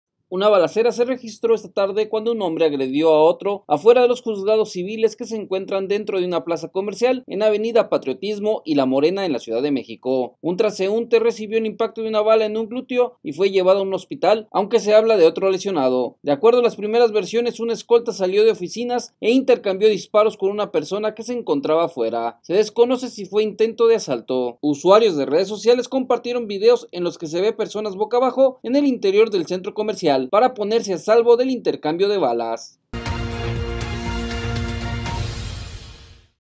Se registró balacera al exterior de Plaza Metrópoli Patriotismo en la Ciudad de México.
Una balacera se registró esta tarde cuando un hombre agredió a otro afuera de los juzgados civiles que se encuentran dentro de una plaza comercial en avenida Patriotismo y La Morena en la CDMX, un transeúnte recibió el impacto de una bala en un glúteo y fue llevado a un hospital, aunque se habla de otro lesionado.
De acuerdo a las primeras versiones, un escolta salió de oficinas e intercambió disparos con una persona que se encontraba afuera. Se desconoce si fue un intento de asalto.